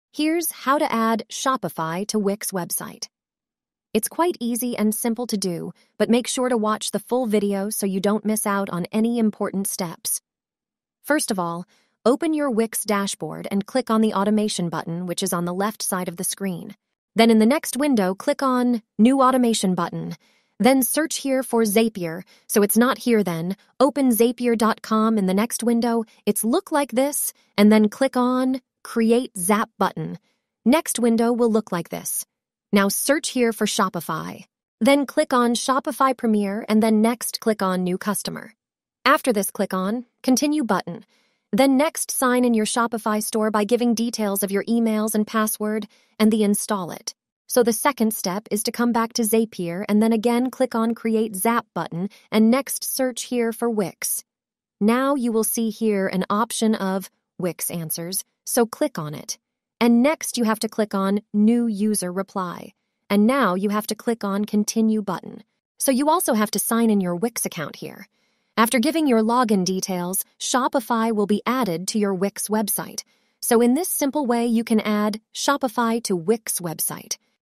CONNECT SHOPIFY TO WIX WEBSITE (QUICK TUTORIAL)
In this video, we'll reveal the secrets to unlock the magic of seamlessly integrating Shopify with your Wix website. If you've ever wondered how to expand your business's potential and elevate your e-commerce game, this is the ultimate solution for you. Keep watching to discover the steps to boost your online store's performance like never before!

0:00 INTRO
0:07 TUTORIAL